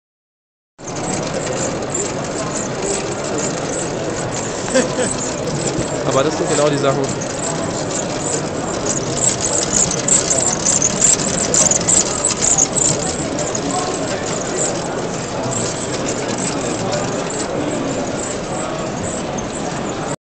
TTeSport Gehäuse Call of Duty mit Gatling Gun & Kühlschrank auf der CeBit
Dieses Gehäuse ist der Hammer!